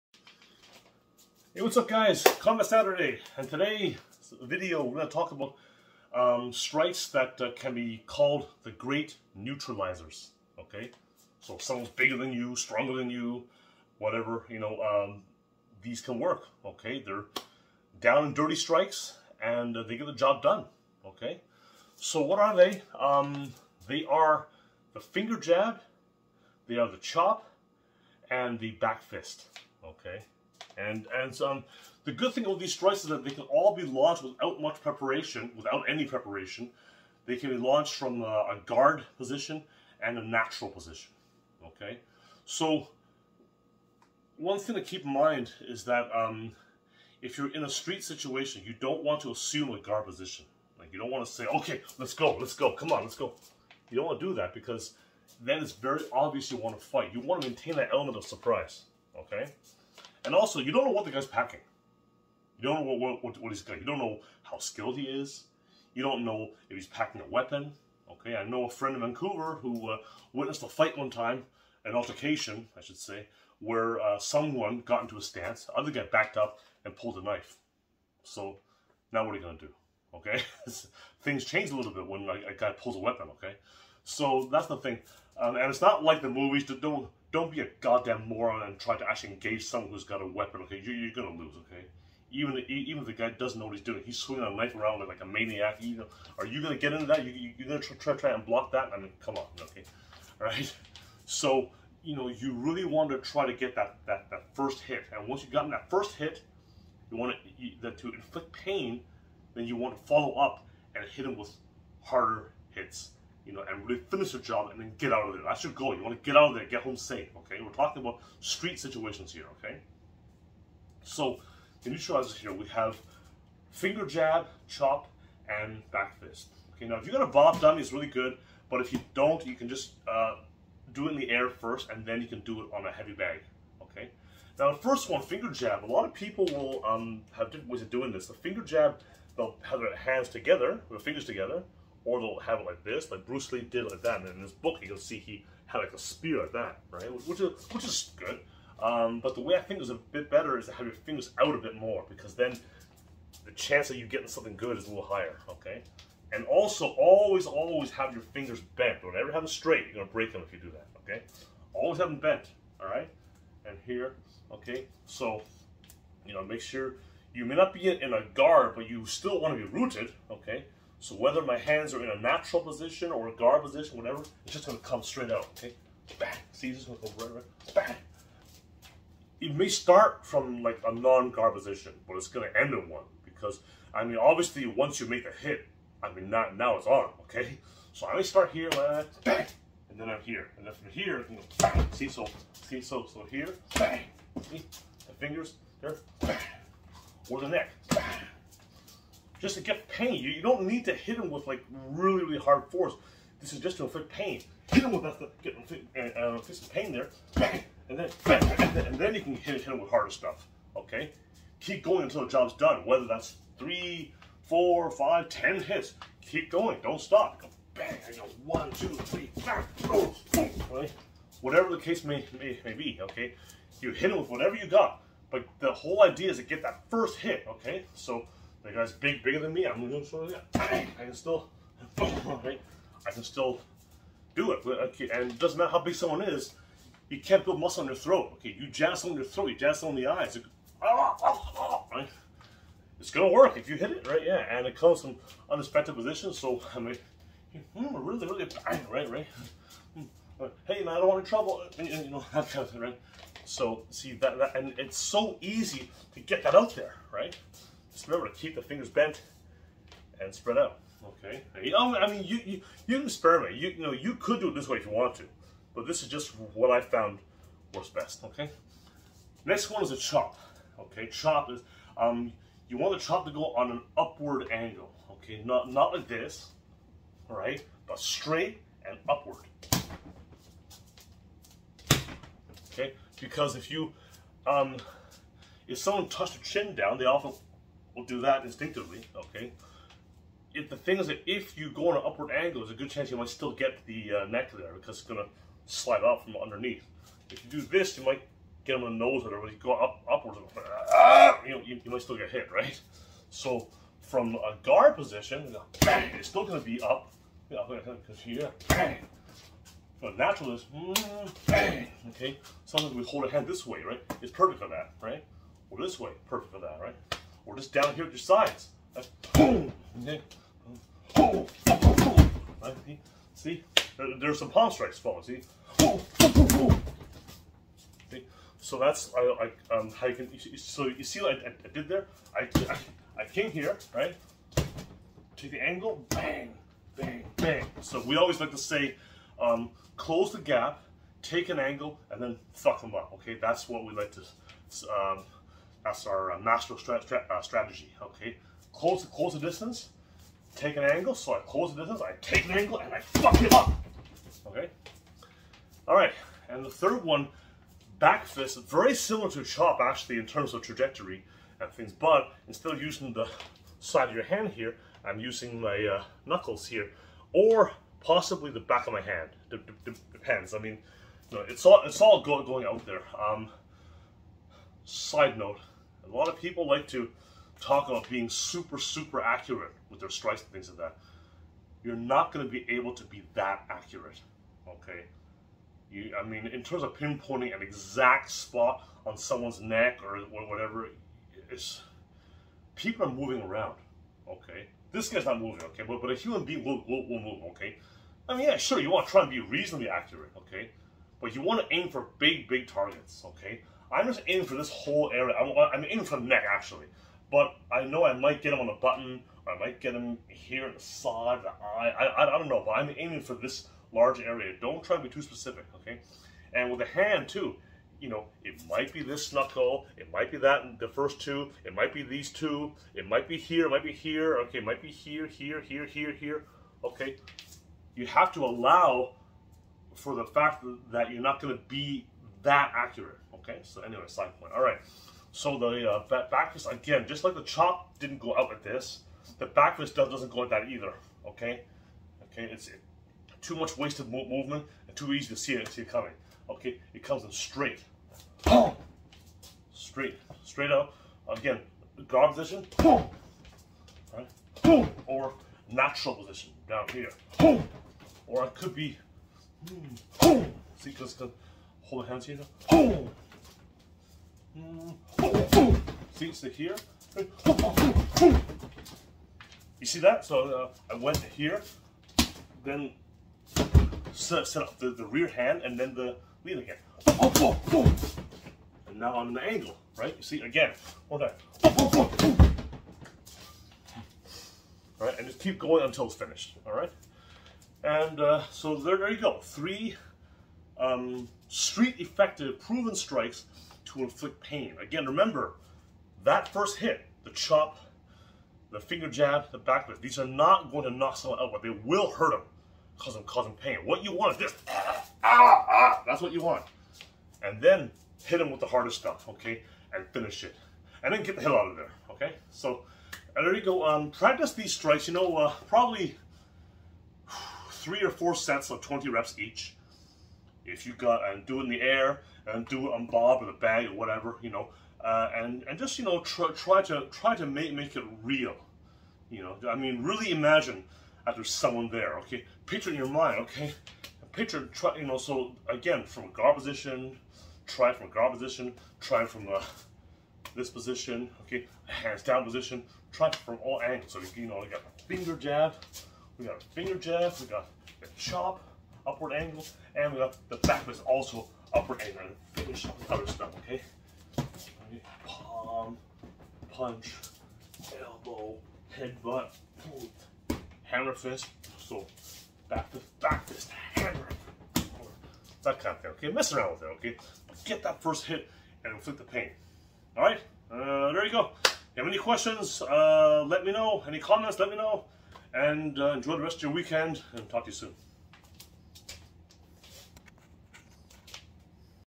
COMBAT SATURDAY - THE GREAT NEUTRALIZERS!
These strikes are designed to inflict pain quickly with minimal preparation. They can be launched from either a guard or natural position. Remember that a self defense situation is very different from a competitive match.